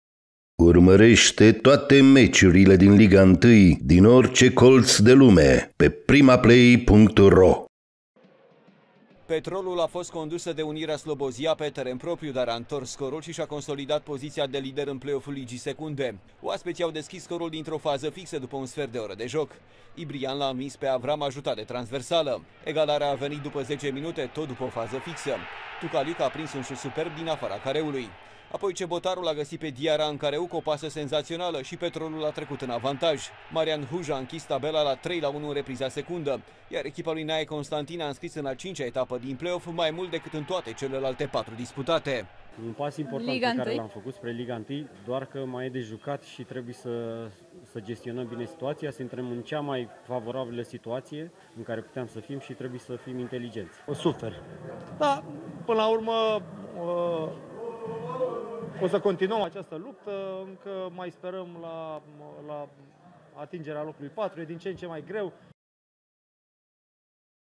REZUMAT | Petrolul Ploiești – Unirea Slobozia 3-1 | Play-off, Etapa 5, Liga 2, 2021-2022
Au marcat: Cosmin Tucaliuc '26, Ibrahim Diarra '37, Marian Huja '49 / Florinel Ibrian '16.